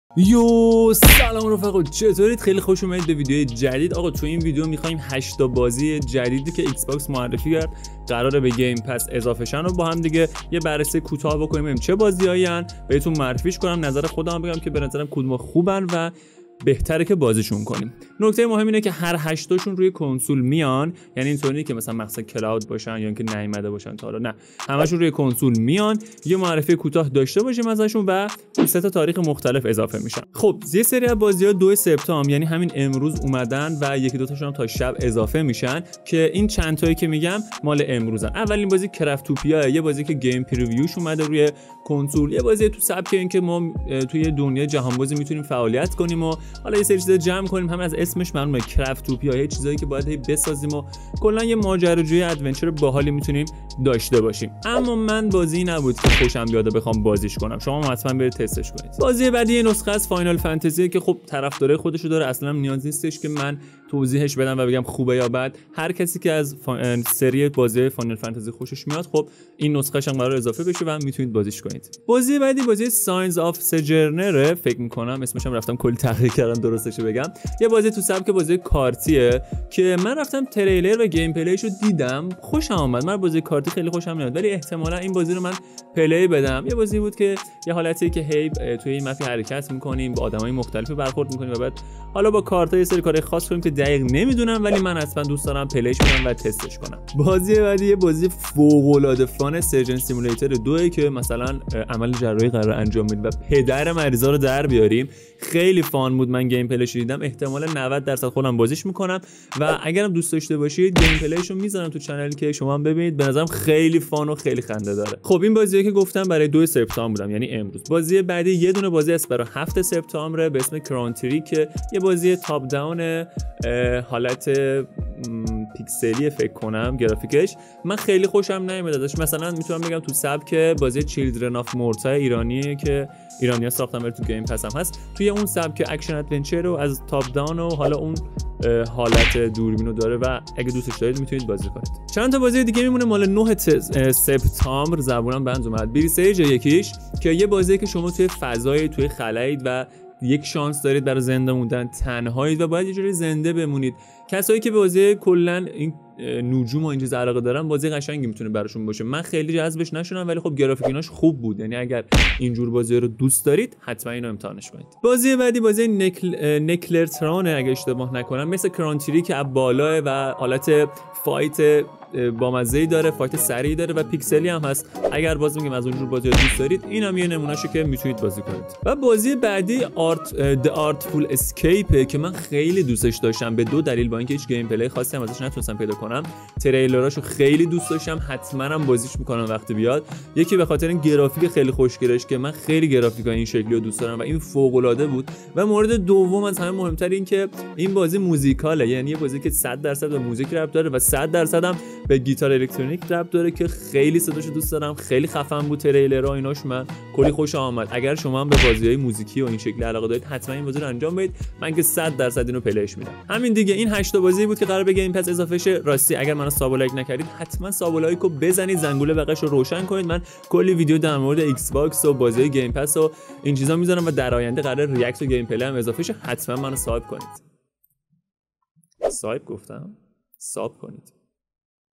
بازی های جدید ایکس باکس | معرفی و بررسی جدیدترین بازی های گیم پس
یوووو سلاطین چطورید؟😍
تو این ویدیو 8 تا از بازی های جدیدی که قراره به گیم پس تو ماه سپتامبر اضافه بشن رو باهم بررسی و تحلیل کردیم  و منم نظر خودمو براتون گفتم

و اینکه سعی دارم از این به بعد به جای هفته ای 3  ویدیو بیشتر براتون بذارم و شما هم حمایت یادتون نره که برای من دلگرمیه❤

خلاصه که عشقید و منتظر خبرای خیلی خفن تر باشید💥❤